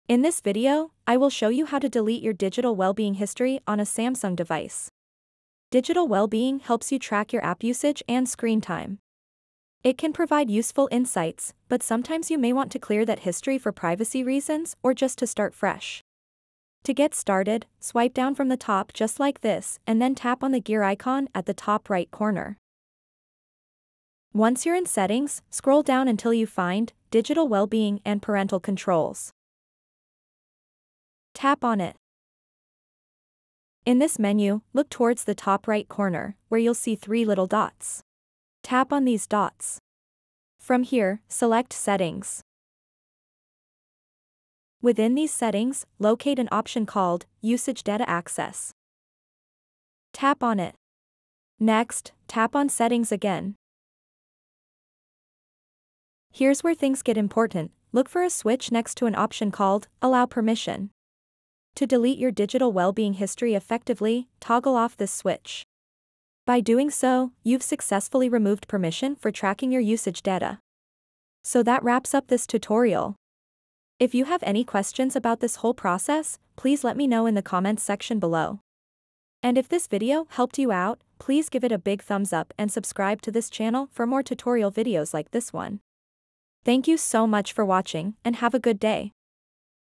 How To Delete Digital Wellbeing History In Samsung
In this video, we will show you How To Delete Digital Wellbeing History In Samsung. Digital Wellbeing is a feature that helps you track your phone usage. It shows how much time you spend on apps and how often you check your device. Sometimes, you might want to clear this data for privacy or to reset your progress.